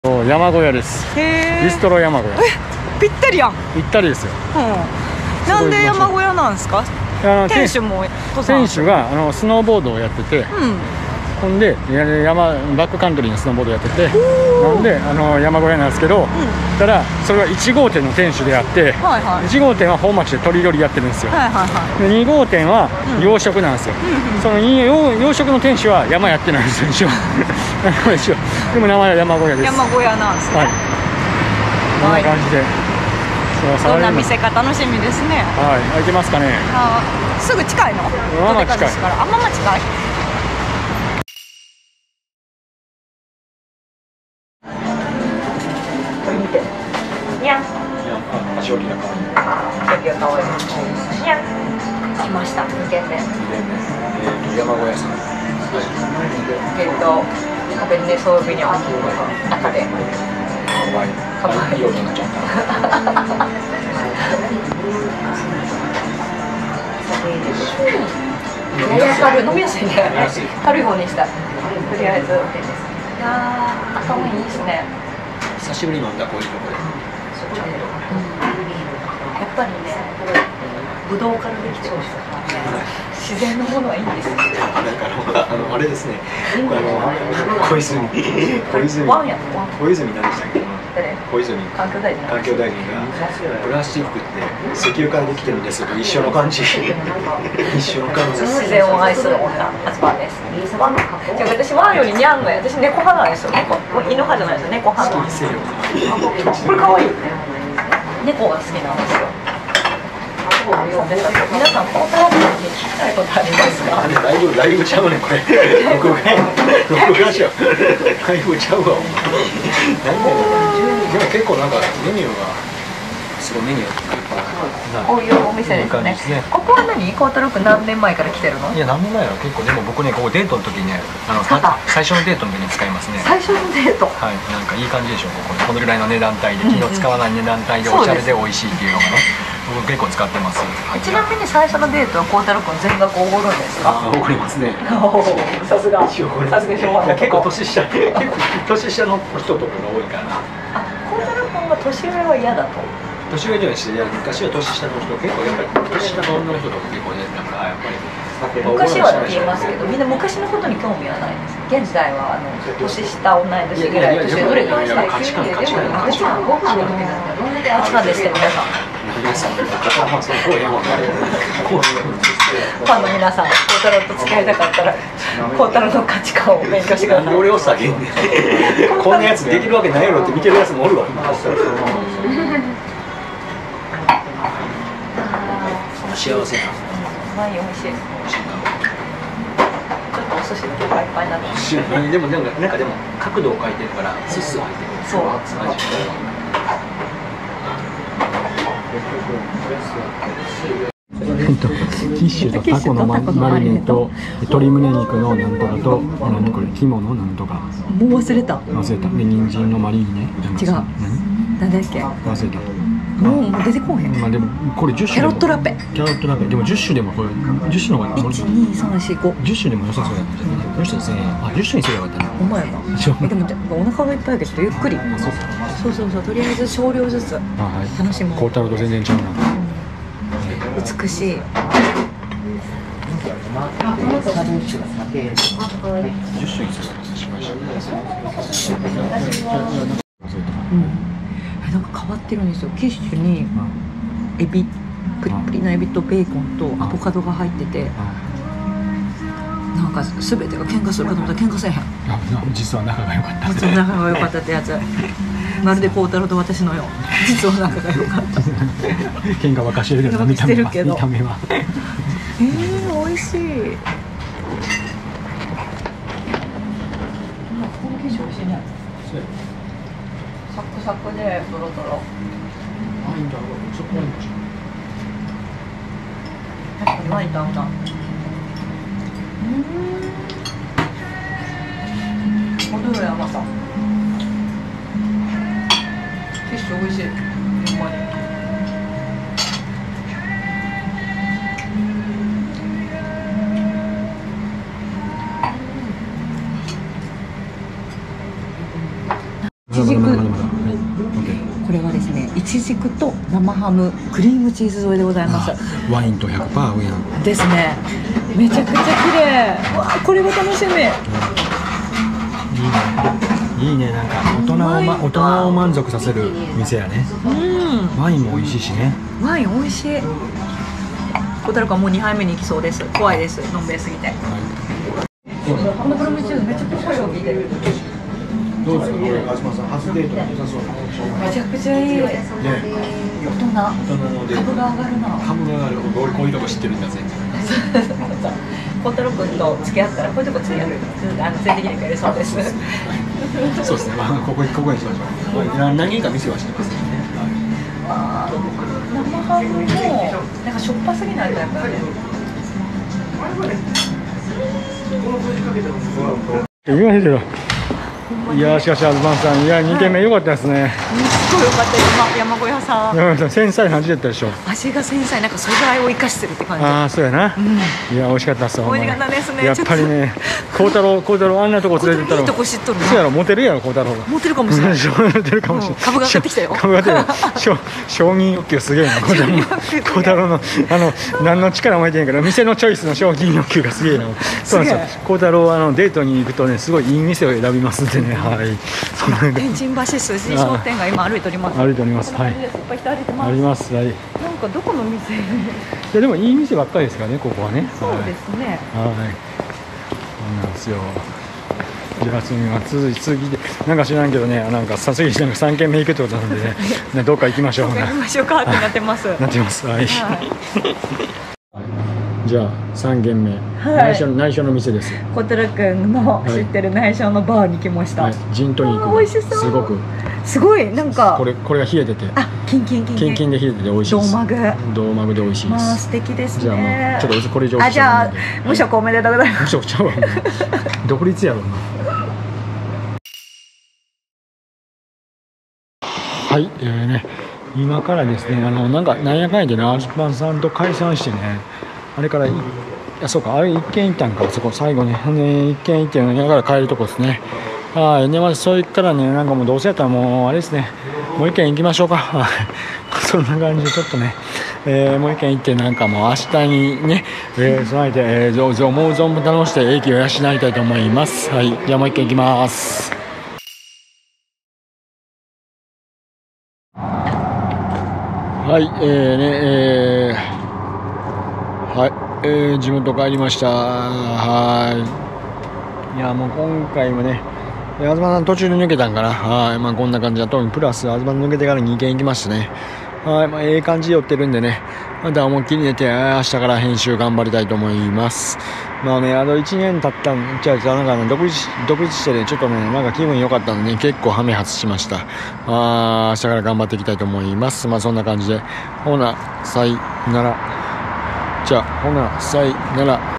あずぱんと南森町界隈ではしご酒！こうたろは５軒はしごしてベロベロになった・・・
オリジナルTシャツ！ステッカー販売してます！